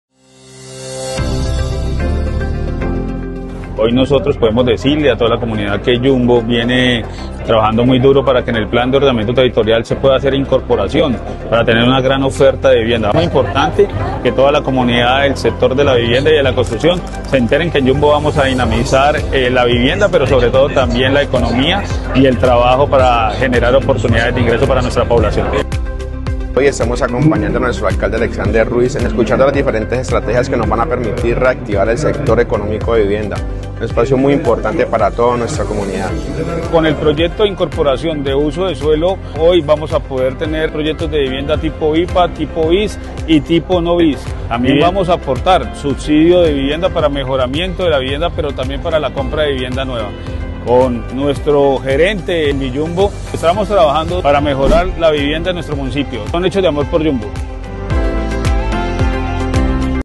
Foro de Ciudad ‘Planes de Desarrollo: acceso a vivienda, metas y desafíos’ Camacol Valle
Vivienda | Yumbo presente en el Foro de Ciudad ‘Planes de Desarrollo: acceso a vivienda, metas y desafíos’ Camacol Valle Un espacio que permite conectar a los sectores públicos y privados por un desarrollo urbano responsable y sostenible, abordando el déficit habitacional y explorando nuevas oportunidades para el sector vivienda de nuestro municipio. 👏🏼👏🏼